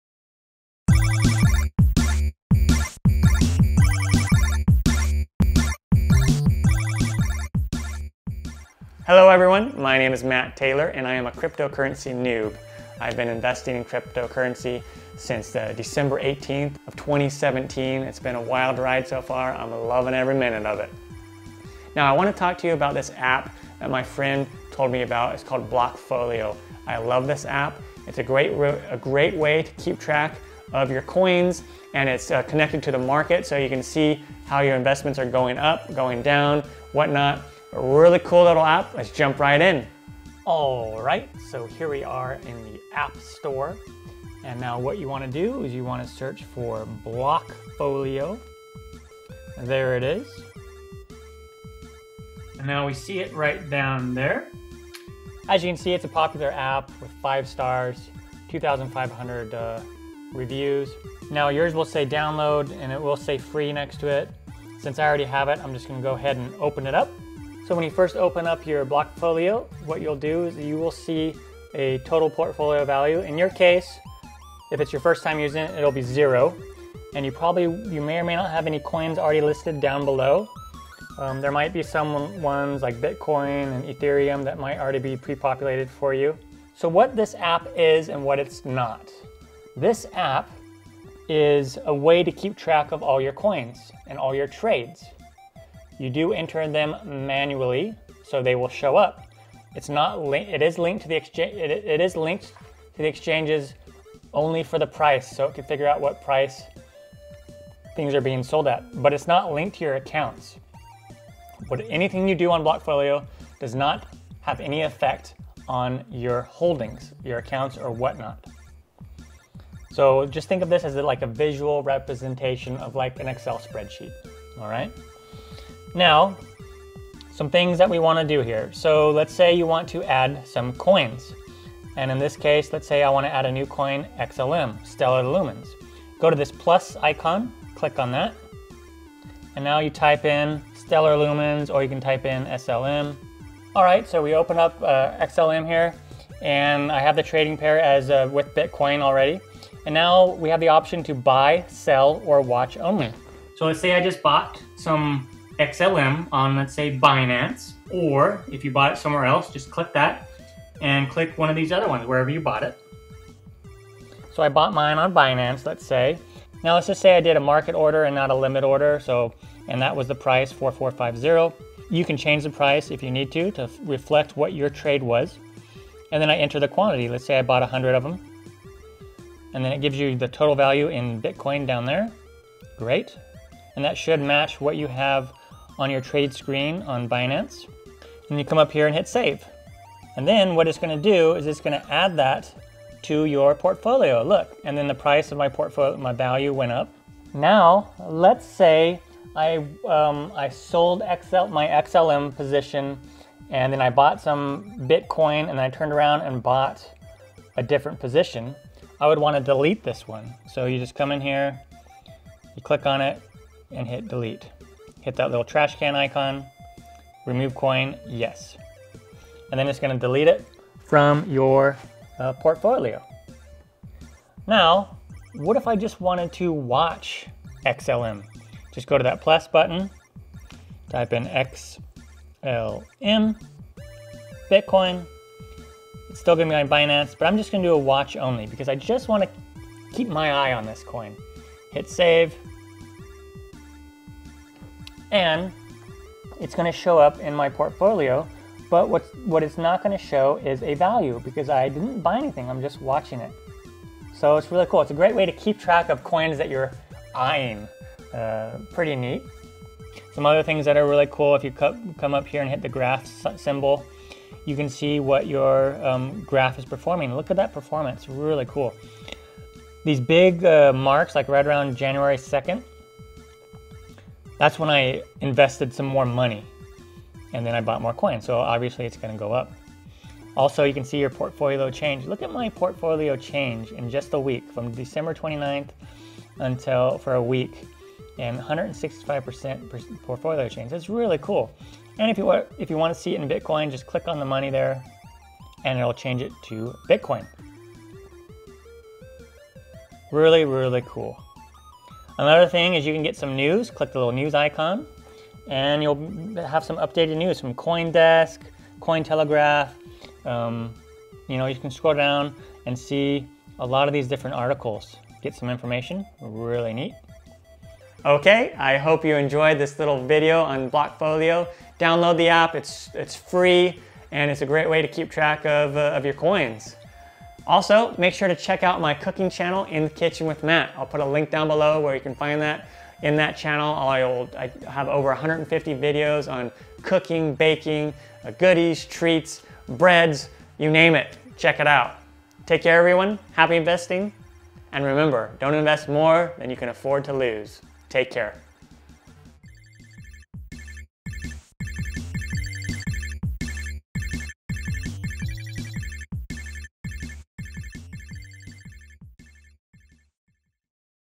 How to Keep Track of Your Cryptocurrency with Blockfolio
In this basic cryptocurrency or digital asset video I will show you the basics on how to use a great application called Blockfolio. Blockfolio is a FREE app that keeps track of the trades you made on various exchanges like binance, coinbase, liqui, cryptopia, bittrex, Kucoin, etc. It doesn't matter if you buy bitcoin, ethereum, ripple, tron, etc. What you do is after you make your trade on the exchange you enter the trade in the app, and since the app is connect to the markets, it gives you a real time look at what your porfolio is worth, and you can add all your trades, regardless of what exchange you traded on. Pretty cool. Blockfolio is NOT connected to your accounts, so if you add or delete anything it doesn't affect your actual holdings.

Make sure to check out my cooking show here on YouTube you can find that